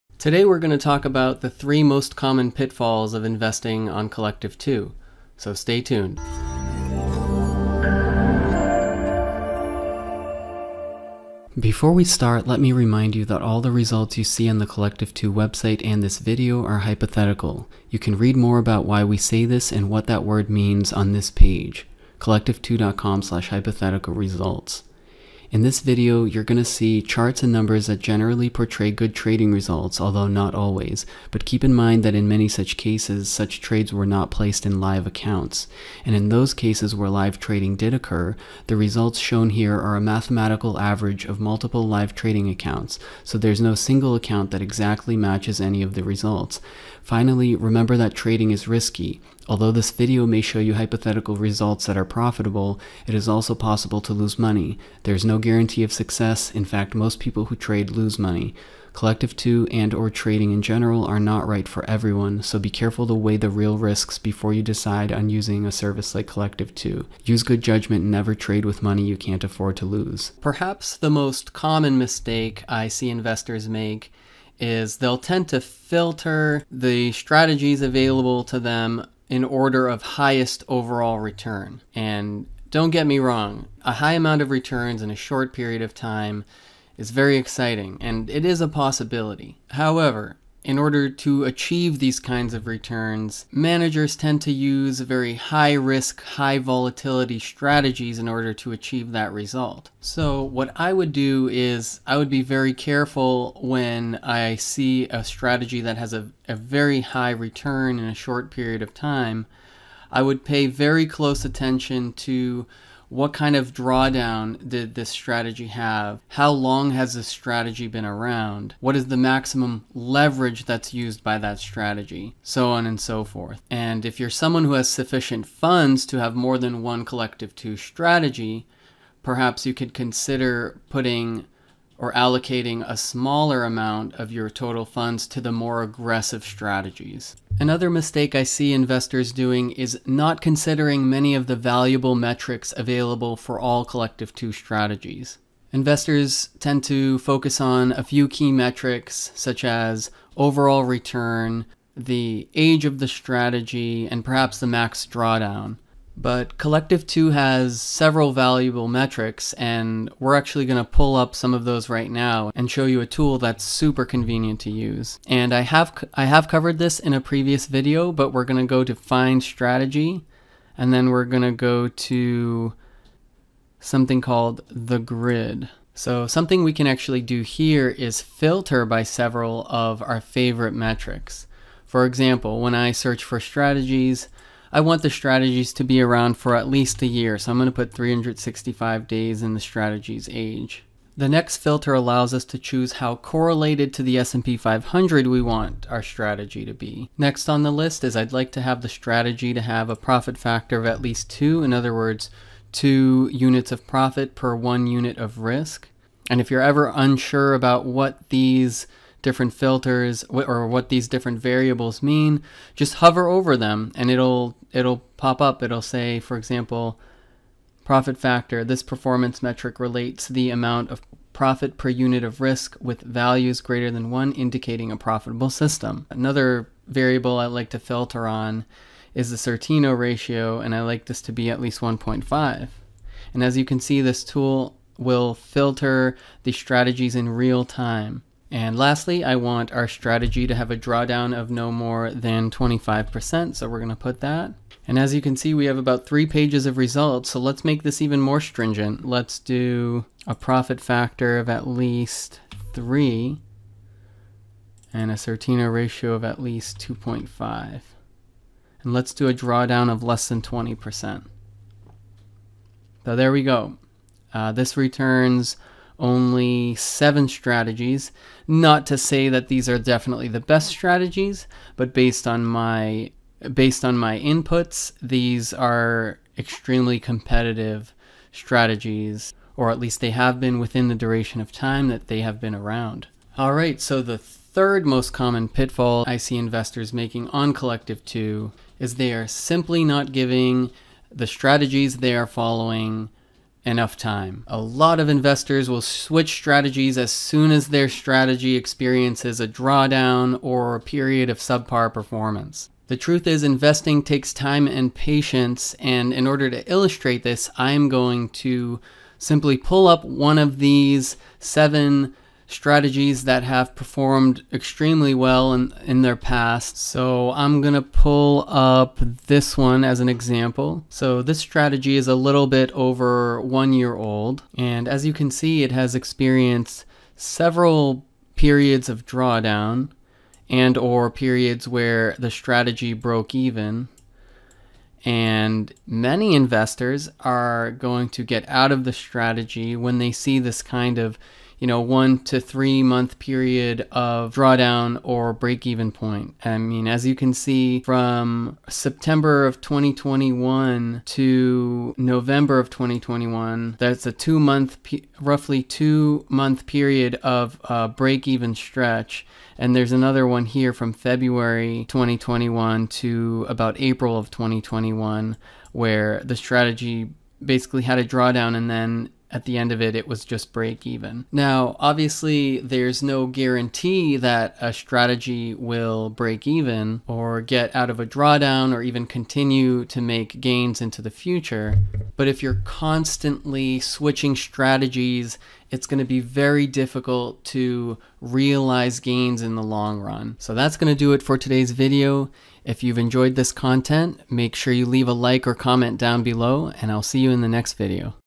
The three biggest mistakes you can make on Collective2
Learn about the three biggest mistakes investors make on Collective2.